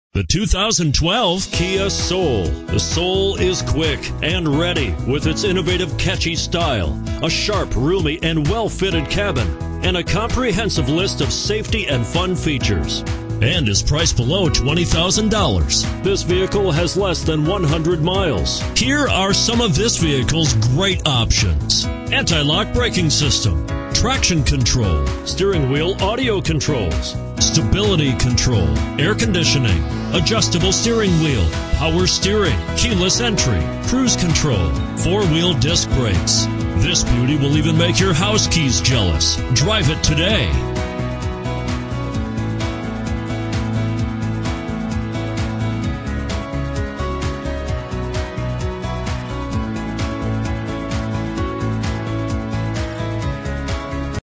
2012 Kia Soul - Station Wagon Winston Salem NC 2KI0090
2012 Kia Soul

Pick up this Molten 2012 Kia Soul, available today at Bob King KIA. Featuring Automatic transmission, it has 4 miles. This could be the one you've been searching for.